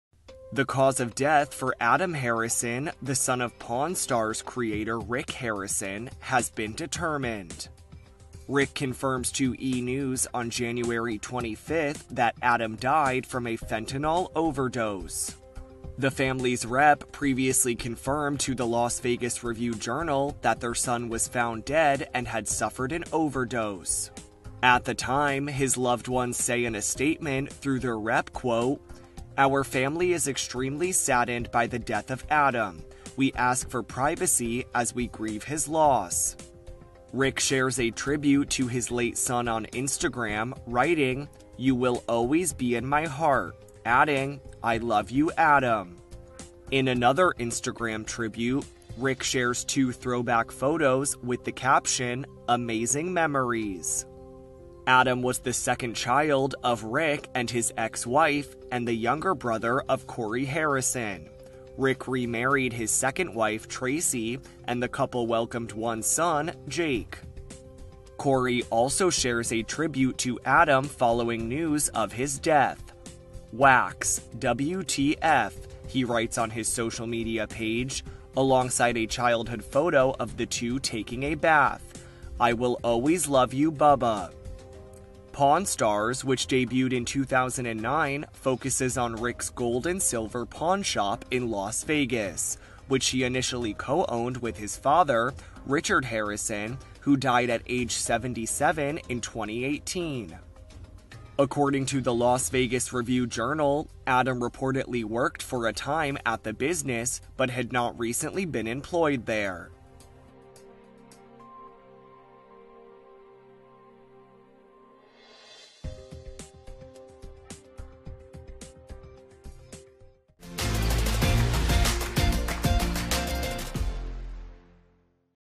Pawn Stars' Rick Harrison’s Son Adam’s Cause of Death Revealed | E! News
Less than a week after Adam Harrison, the son of 'Pawn Stars' host Rick Harrison, was found dead, new details about his passing have been revealed. Adam, who Rick shared with ex-wife Kim Harrison, was found dead on Jan. 19. Adam was the second-eldest of Rick's three sons.